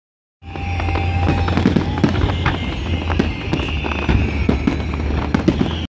Sunfest fireworks - West Palm Beach
A nice end to another Sunfest  in West Palm Beach, Florida.